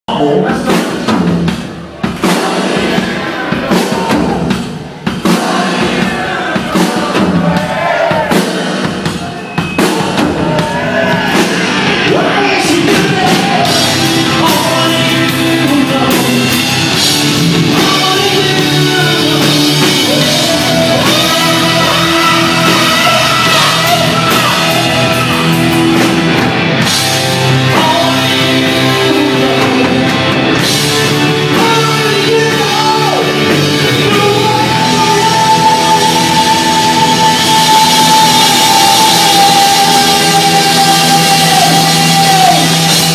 CRYER AT THE ASYLUM JULY 10 - STU CLARKE MEMORIAL SHOW (2)
VIDEO 2 OF 3 FROM STU CLARKE TRIBUTE NIGHT.

CRYERS FIRST SHOW FOR 28 YEARS !